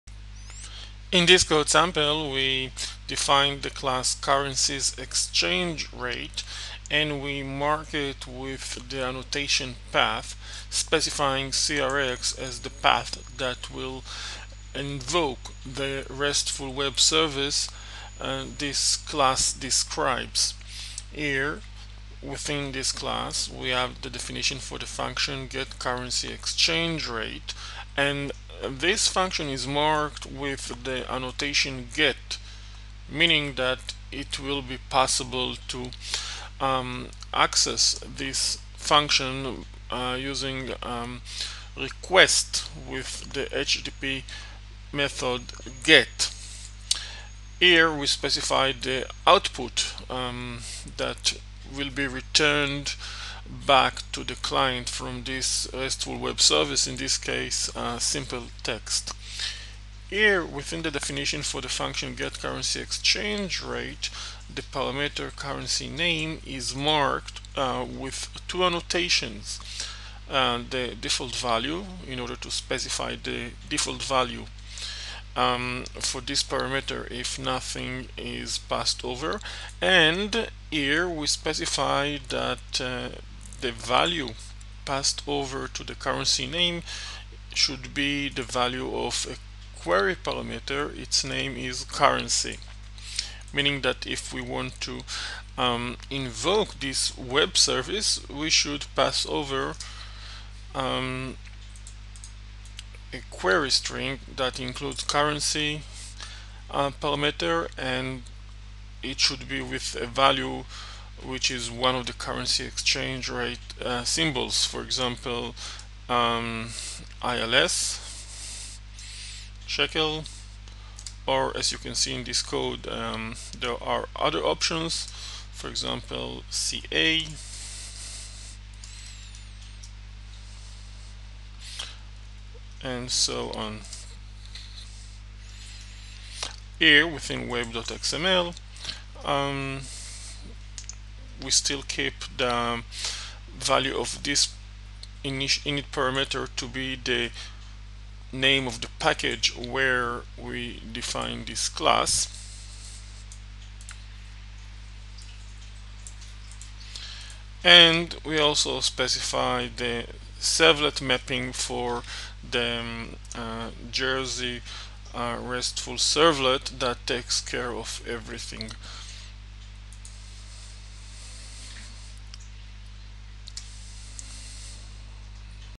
Simple REStful Web Service with Parameters using Jersey
This is a short video clip that shows how to develop a simple REStful web service that receives parameters. This video clip uses the Jersey open source reference implementation for JSR 311.